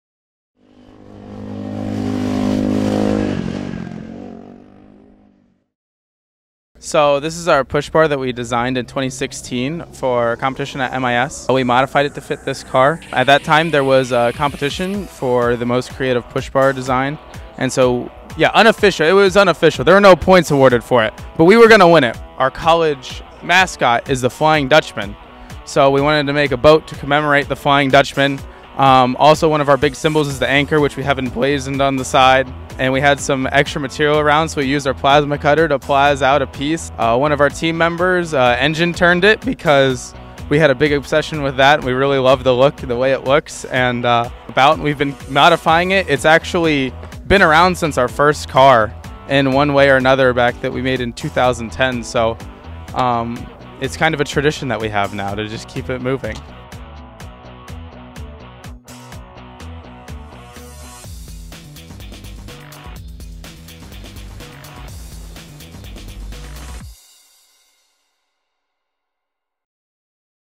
Choosing "The Look": Push Bar Edition
In this video, we visit the paddock of Hope College to take a look at their push bar, which has become part of their team's Formula SAE legacy.